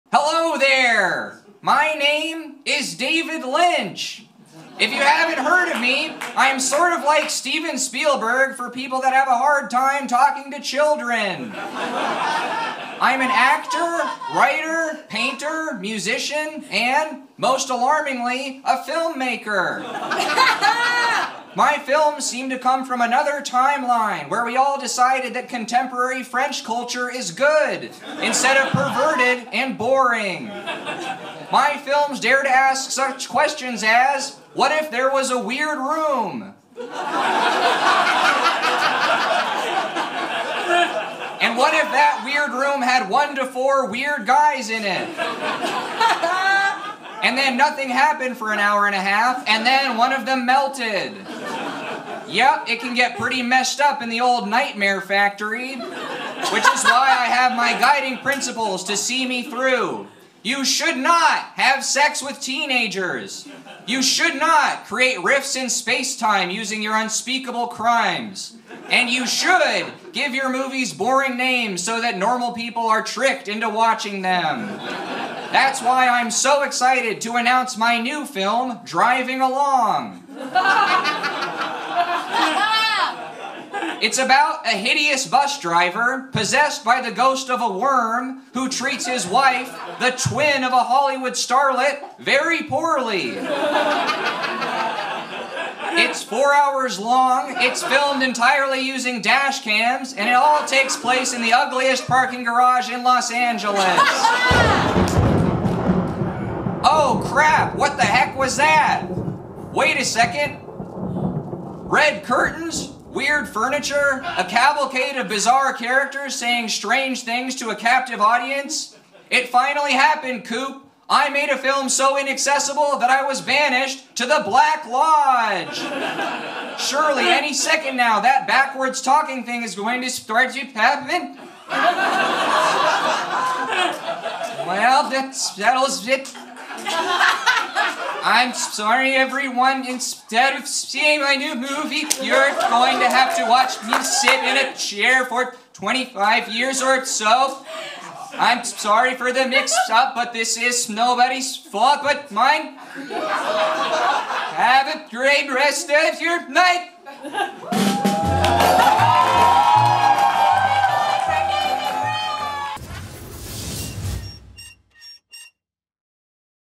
David Lynch Introduces Himself To The Funny Or Die Lab
Filmmaker David Lynch (David Brown) joins The Funny Or Die Lab to talk about his whole deal.